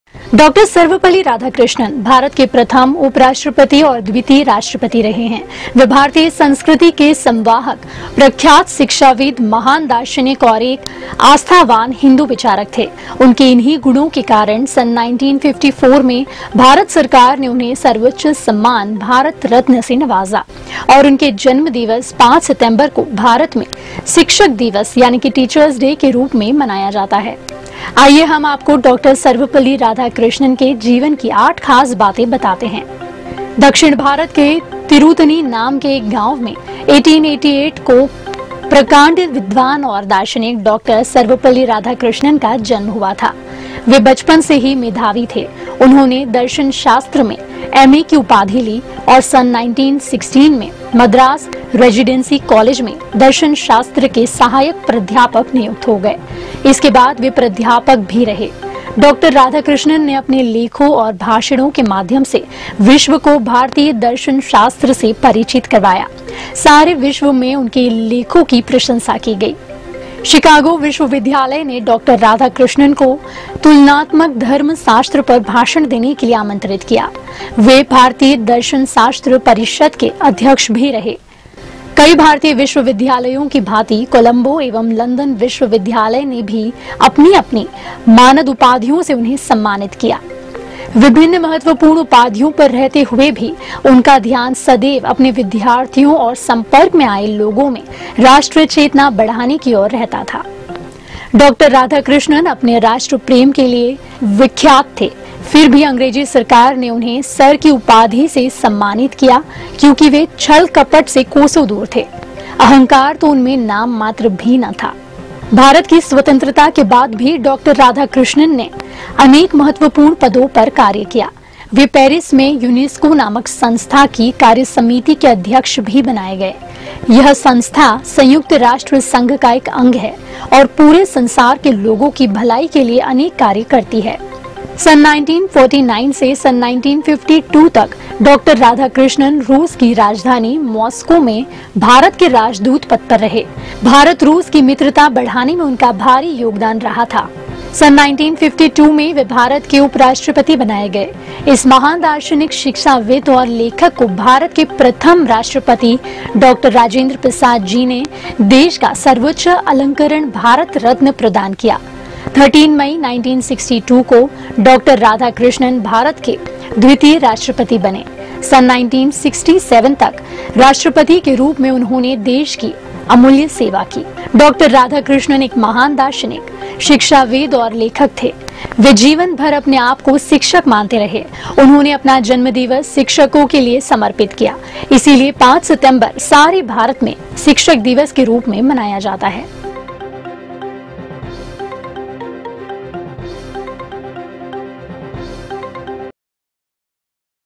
शिक्षक दिवस पर जरूर जानिए Dr. Sarvepalli Radhakrishnan की 8 खास बातें
डॉ. सर्वपल्ली राधाकृष्णन भारत के प्रथम उप-राष्ट्रपति और द्वितीय राष्ट्रपति रहे है। वे भारतीय संस्कृति के संवाहक, प्रख्यात शिक्षाविद, महान दार्शनिक और एक आस्थावान हिन्दू विचारक थे। उनके इन्हीं गुणों के कारण सन् 1954 में भारत सरकार ने उन्हें सर्वोच्च सम्मान 'भारत रत्न' से नवाजा और उनके जन्म दिवस (5 सितम्बर) को भारत में शिक्षक दिवस (teachers day) के रूप में मनाया जाता है।आइए, हम आपको डॉ. सर्वपल्ली राधाकृष्णन के जीवन की 8 खास बातें बताते हैं